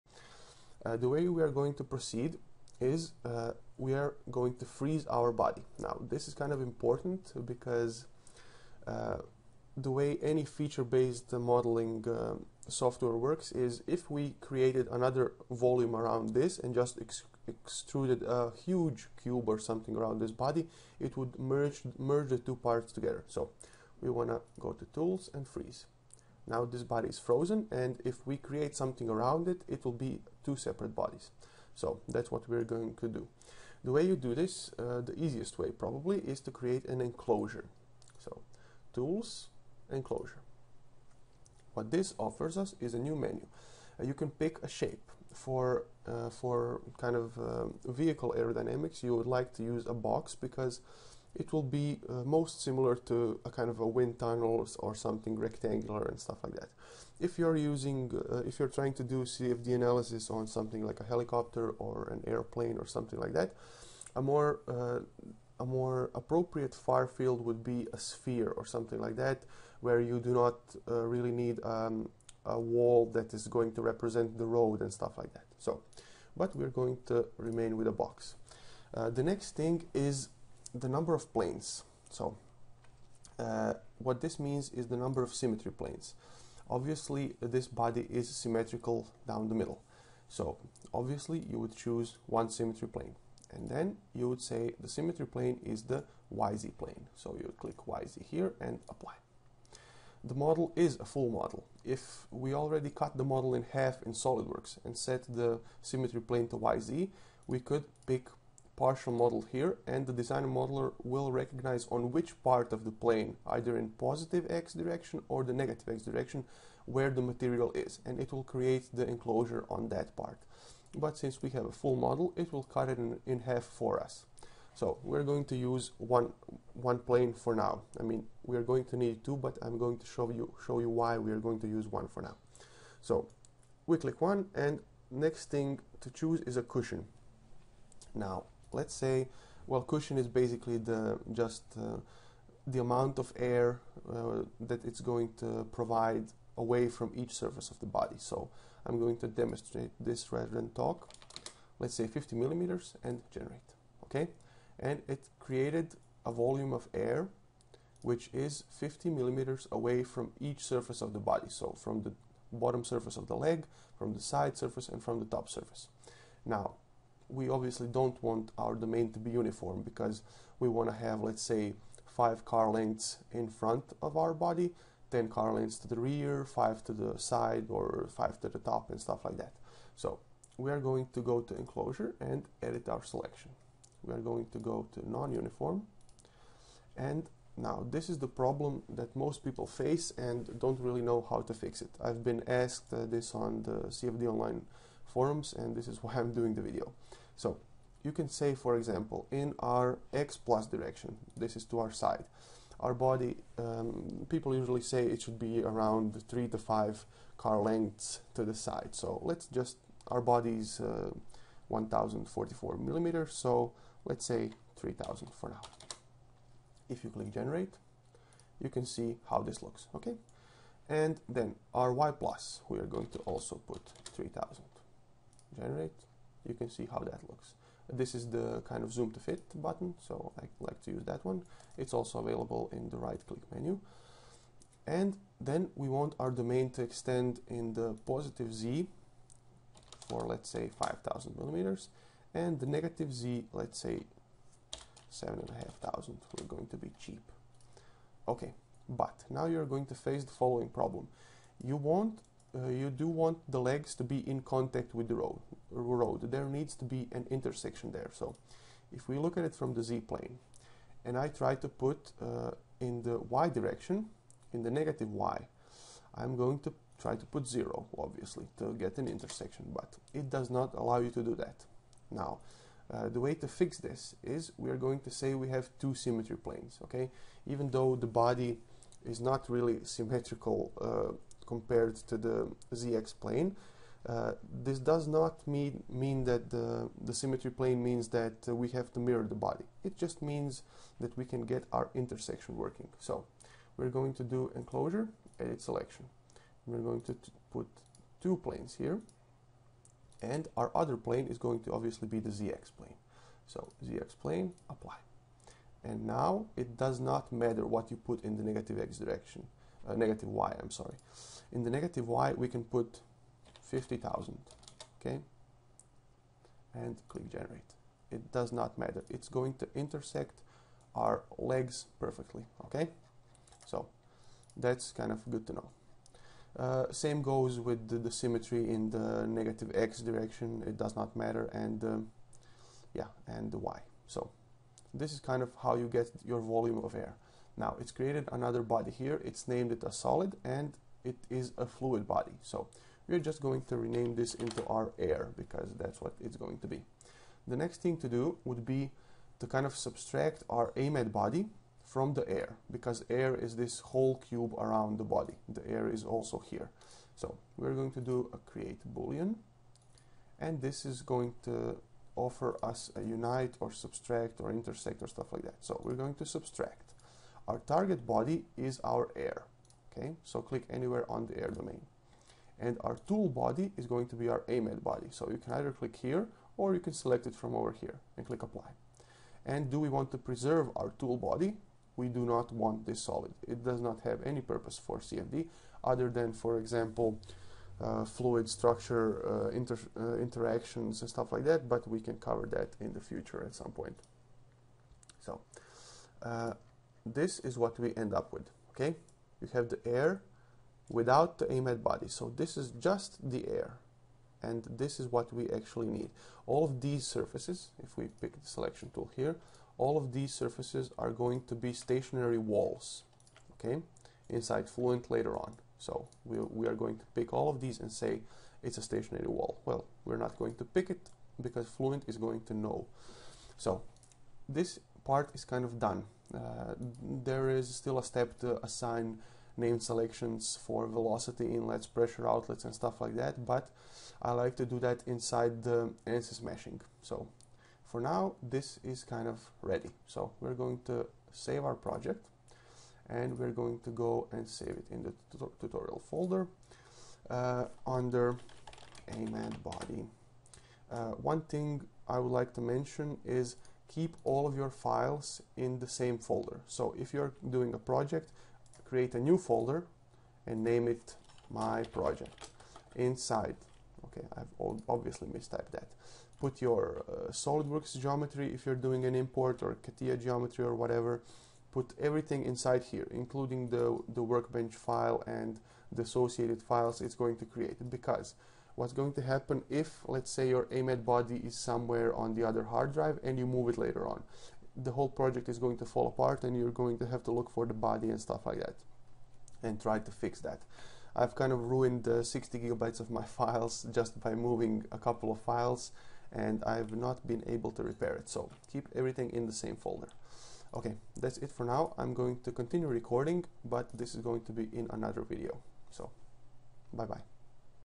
Part 1b - DesignModeler for Vehicle Aerodynamics
Continuation of part one of the series of videos guiding you through CFD usage in external vehicle aerodynamics with ANSYS Workbench 13. Parts are going to be:

1. Geometry preparation using DesignModeler.
2. Mesh generation using ANSYS Meshing.
3. Case setup, solving and post-processing in ANSYS Fluent.
4. Post-processing and visualisation in ANSYS CFD-Post.

I'm sorry that the first part is split into a/b but it turned out 17 minutes long with me skipping things along the line. I will create a playlist of all files to make it easier on you guys.